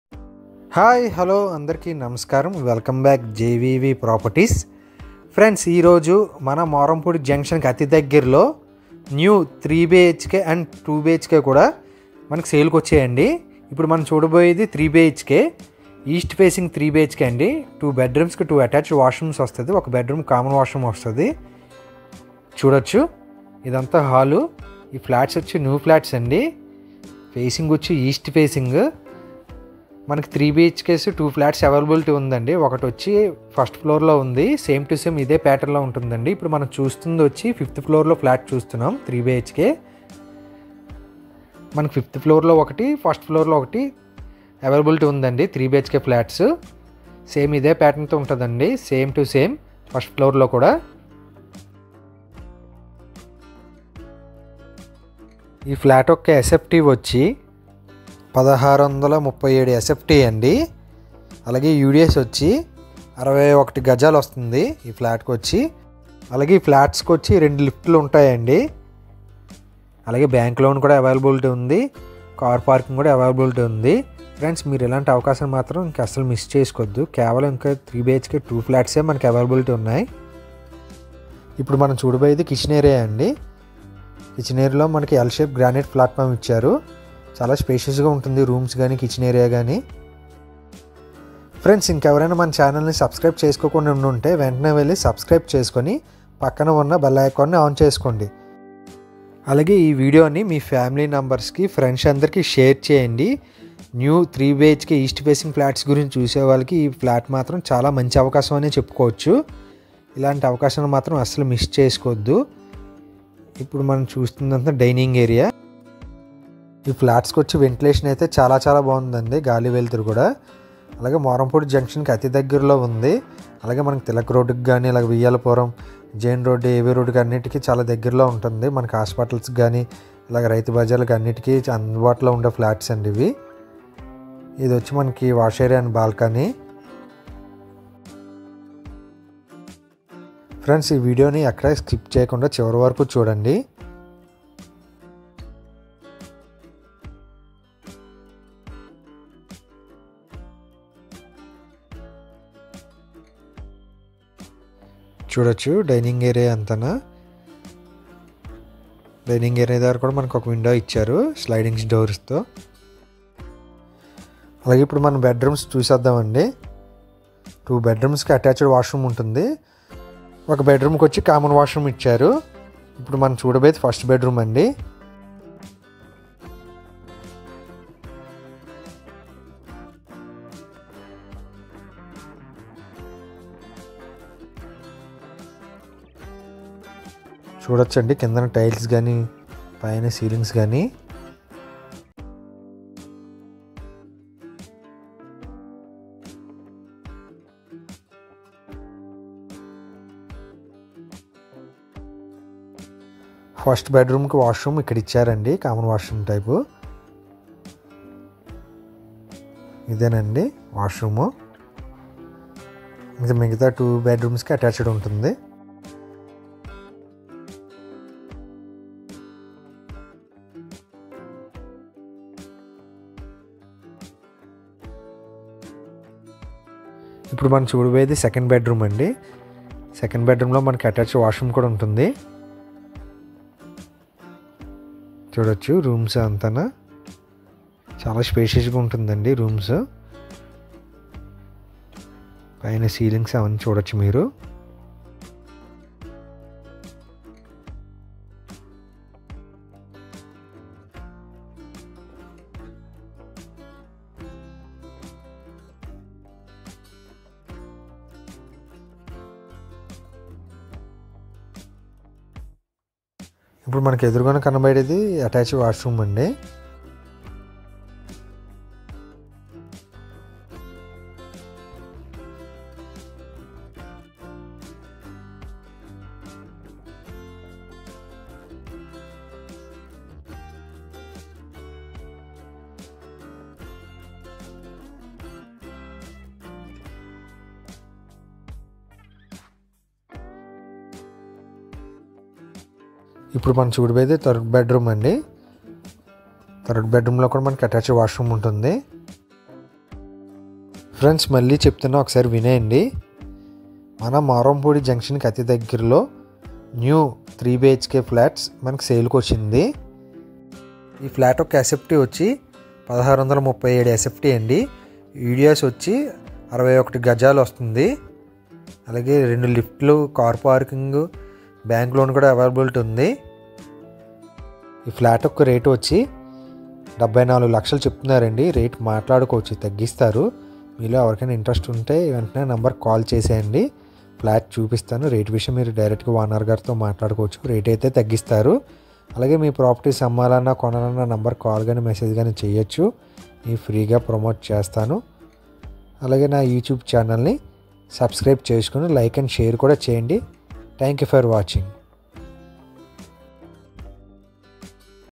మోరంపూడి ఈస్ట్ ఫేసింగ్ న్యూ 3 BHK ఫ్లాట్ 74 లక్షలు 1637 sft uds 61 5th floor.1st floor available
రాజమండ్రి

మోరంపూడి జంక్షన్ అతి దగ్గరలో  న్యూ 3 BHK flat  for sale

new flats

3 BHK

east facing          1st floor and  5th floor  available

1637 sft

uds 61

2 lifts available

bank loun available

74 లక్షలు చెప్తున్నారు రేట్ మాట్లాడుకోవచ్చు తగిస్తారు

j v v propertys rajahmundry